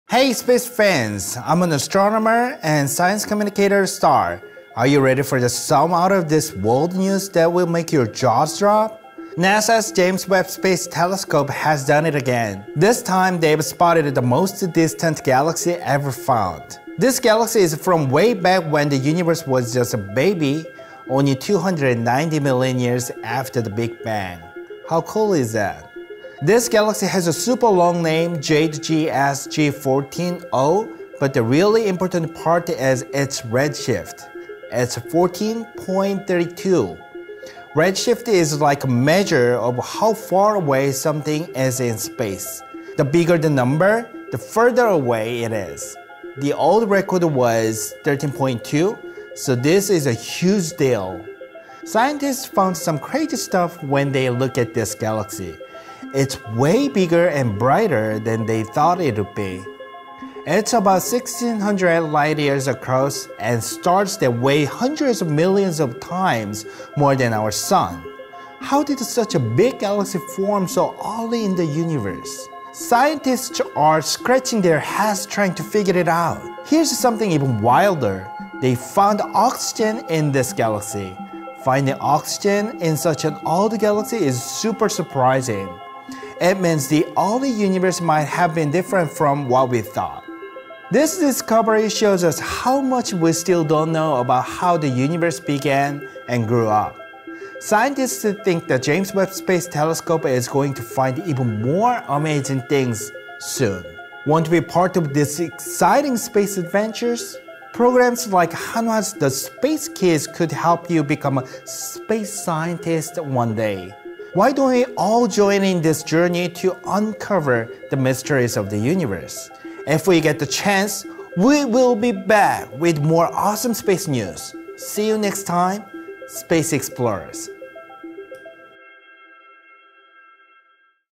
[EN] James Webb just did it again
NASA's James Webb Space Telescope has done it again!
This time, they've spotted the most distant galaxy ever found.
Can you believe it? This galaxy is from way back when the universe was just a baby,
only 290 million years after the Big Bang! How cool is that?

NASA의 제임스 웹 우주 망원경이 또 한 번 역사를 새로 썼습니다.
이번에는 지금까지 발견된 것 중 가장 먼 은하를 찾아 냈다고 하네요.
이 발견은 우주의 아주 초기, 빅뱅 이후 겨우 2억 9천만 년밖에 지나지 않은 시기의 은하를 관측한 거예요.
얼마나 대단한지 상상이 가시나요?